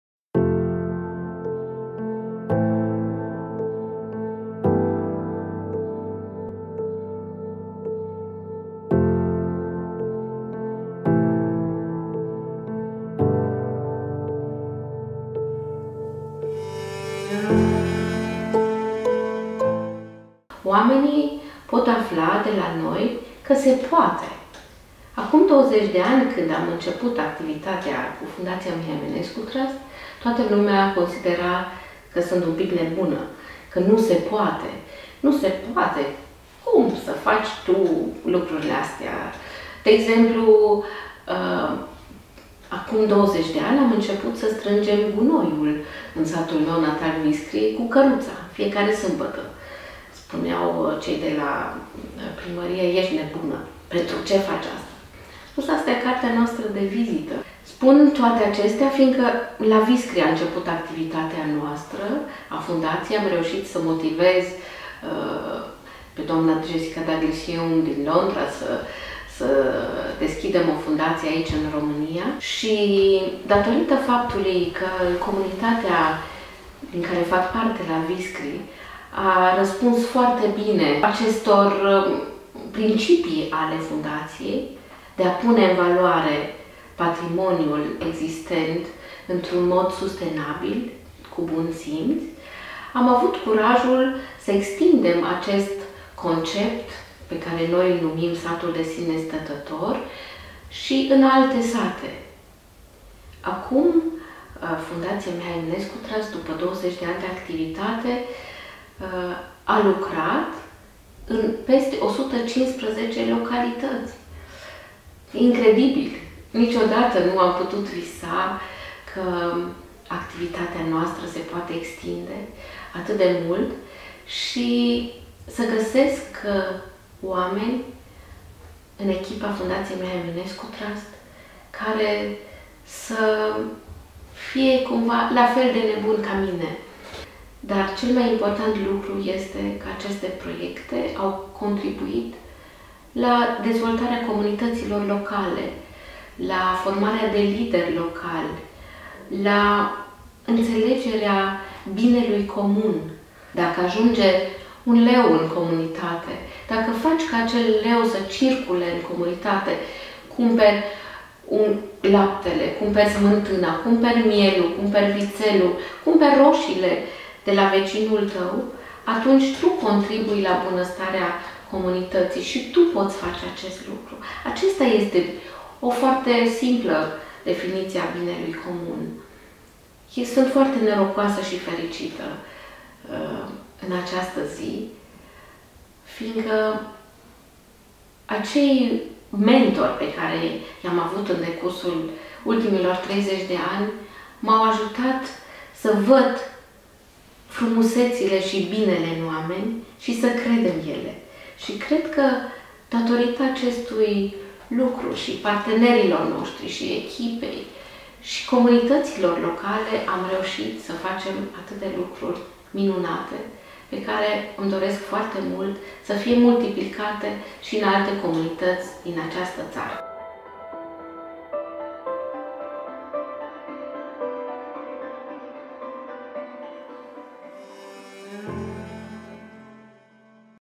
Aniversare Mihai Eminescu Trust - 20 de ani de activitate - Caroline Fernolend, Președinte
Caroline Fernolend, Preşedintele Mihai Eminescu Trust, vorbește despre principiile și realizările fundației, la 20 de ani de la înființarea acesteia în România.
Materialul video este realizat de Cezar Machidon.